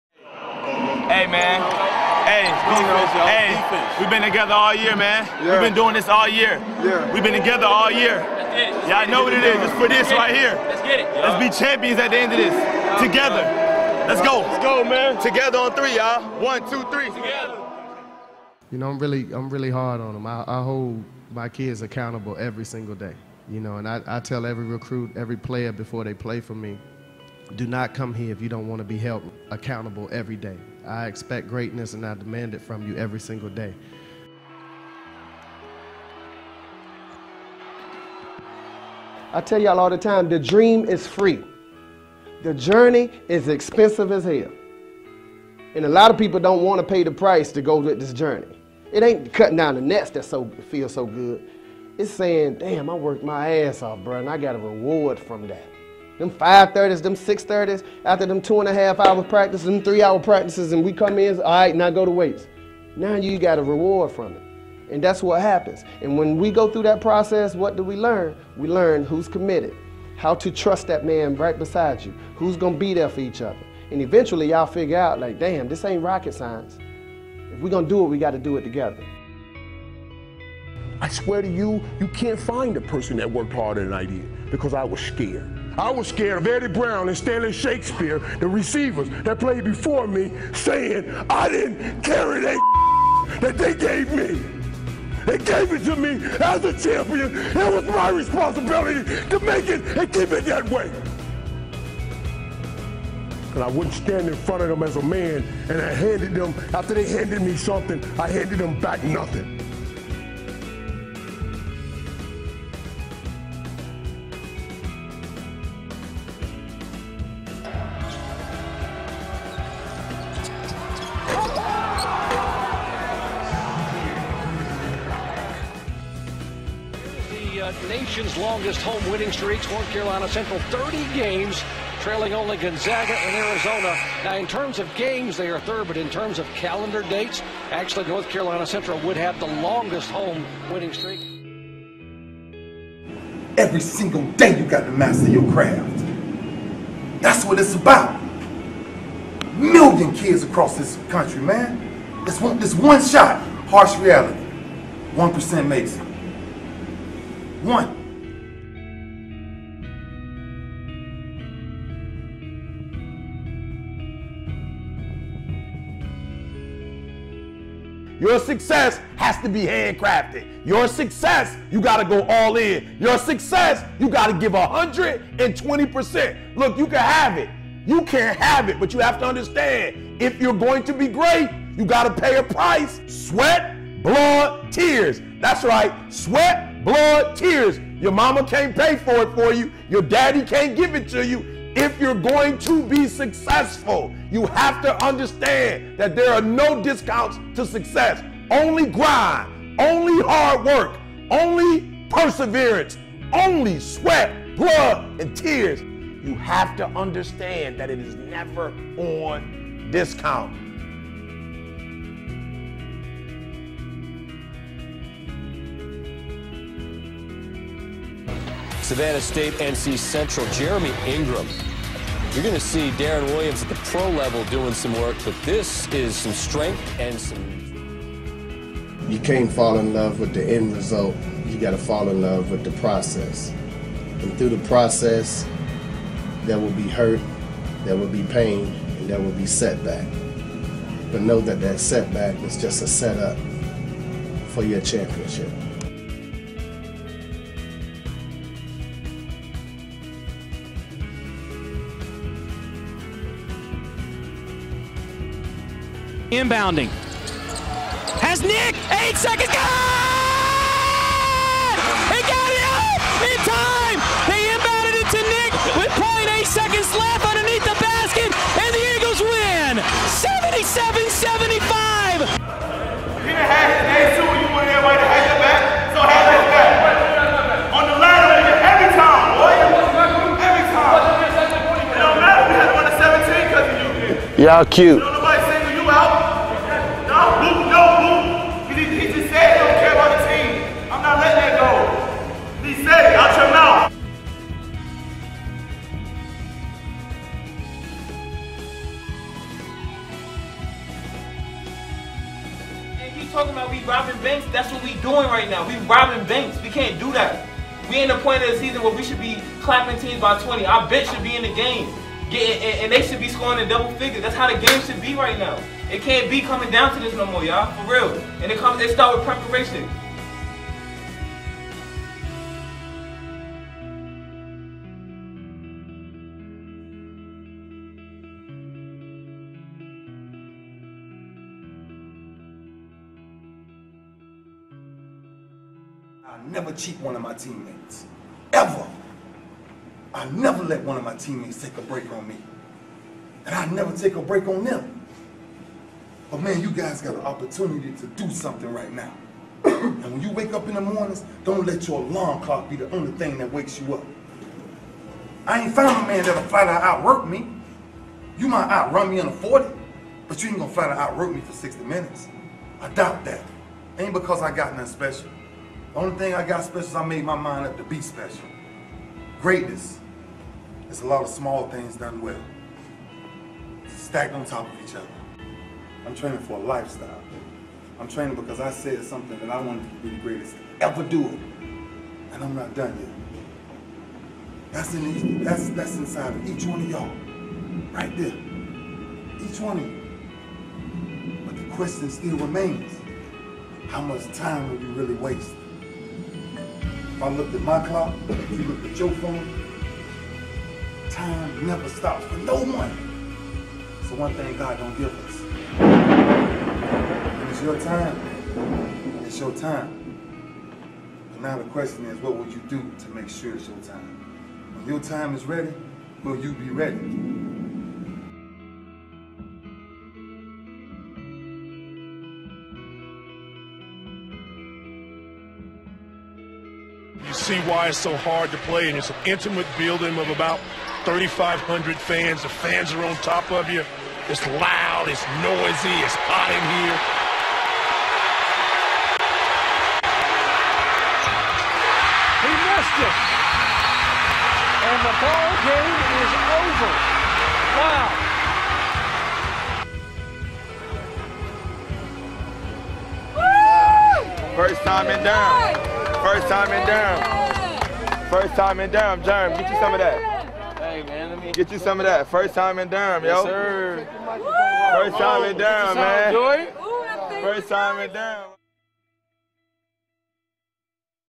North Carolina Central Men's Basketball "Swagger"
As the North Carolina Central University men's basketball team prepares for their home opener, relive their past achievements in this video put together by men's basketball video coordinator Derrick Mallison.  The Eagles open at home on Monday, Nov. 16 against Allen at 8:00 p.m.